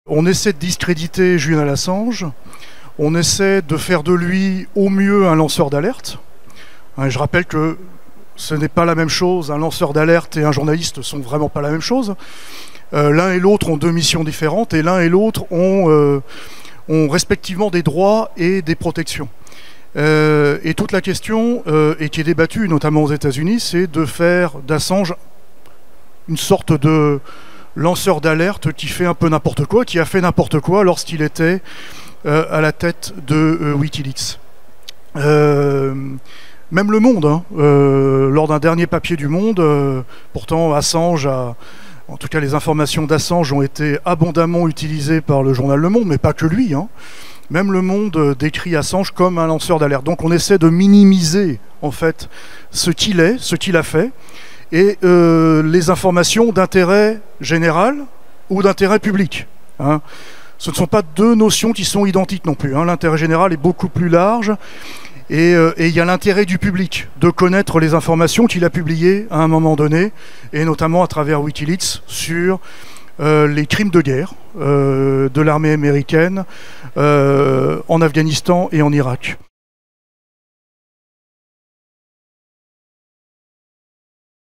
Assange n'est pas un lanceur d'alerte. C'est un journaliste ! - Antony Bellanger
_Antony Bellanger, secrétaire général de la Fédération Internationale du  Journalisme